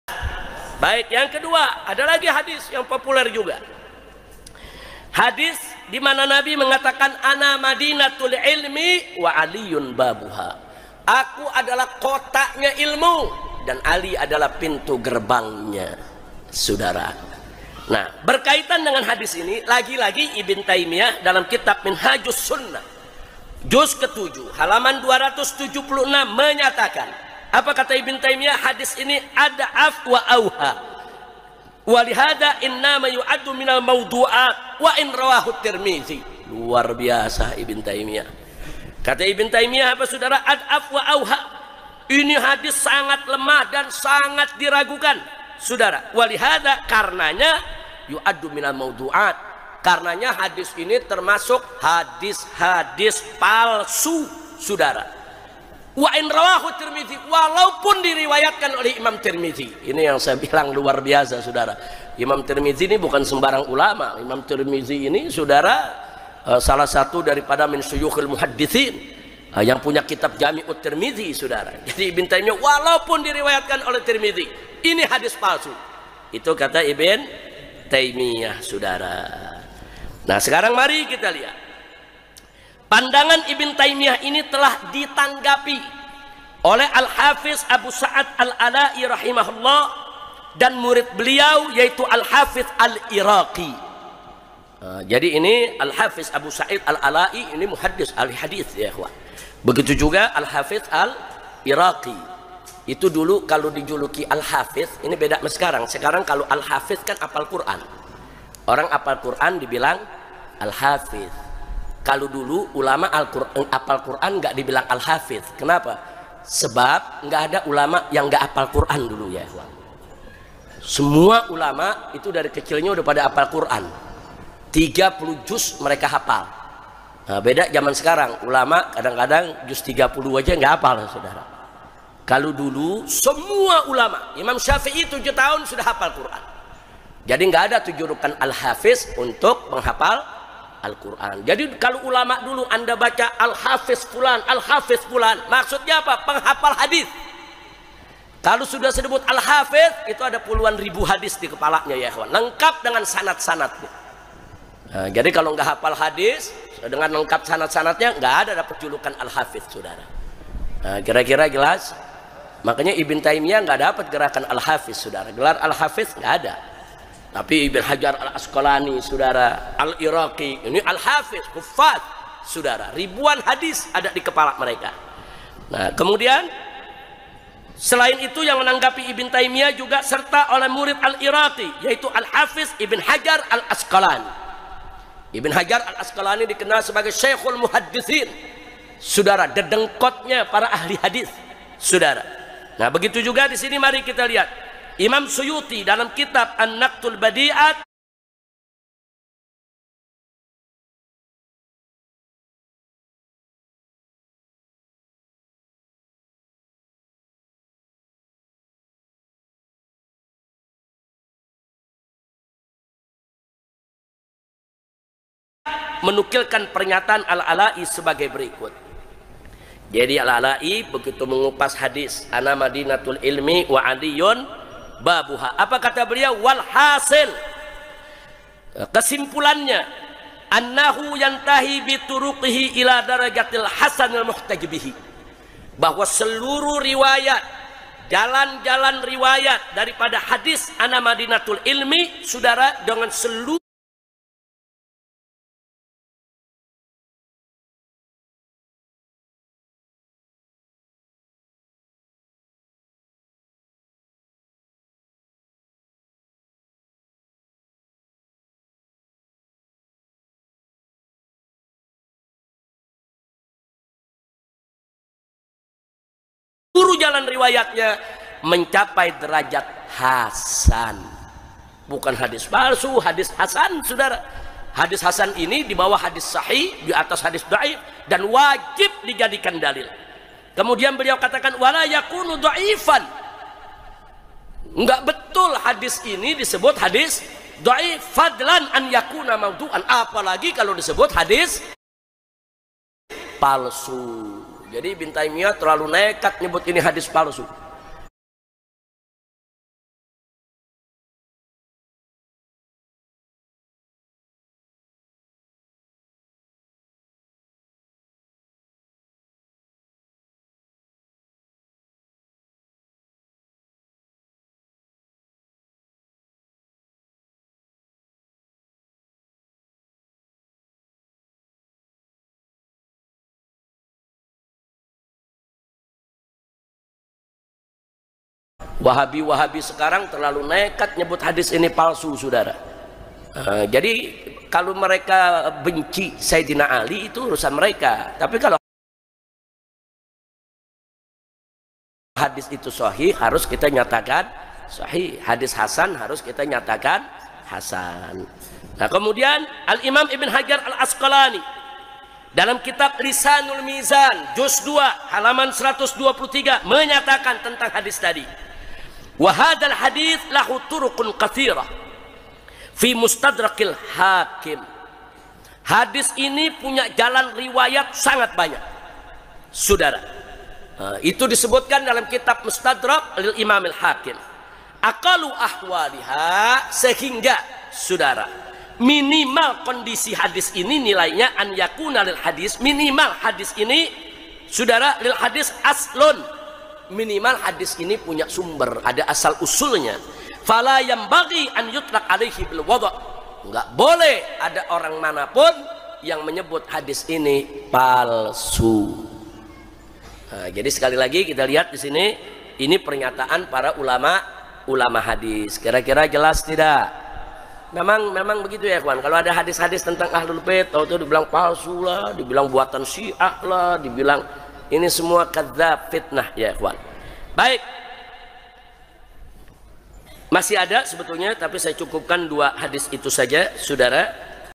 HABIB RIZIEQ SYIHAB BONGKAR FITNAH DEDENGKOT WAHABI
PESAN HABIB RIZIEQ SYIHAB BAHAYANYA KELOMPOK WAHABI  PEMBELOKAN DALIL WASILAH

HABIB RIZIEQ SYIHAB TURUN TANGAN MENJAWAB PERSOALAN NASAB. SADDAH BA'ALAWI || NASAB WALI SONGO

PESAN HABIB RIZIEQ SYIHAB HRS || HATI HATI TERHADAP BEGAL NASAB || NASAB HABAIB BAALAWI